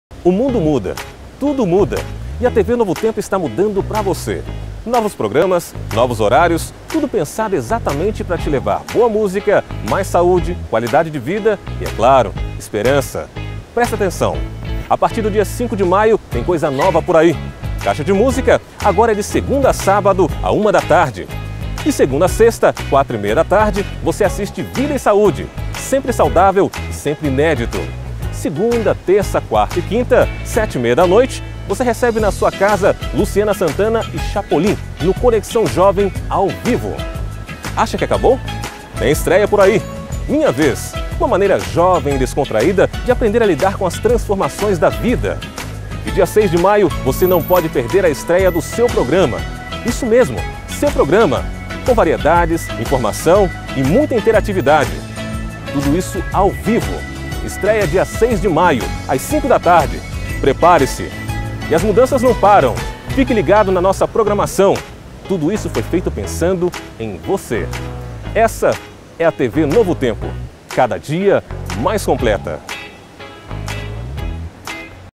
Nova programação Novo Tempo 2013
Confira a nova programação da TV Novo Tempo para 2013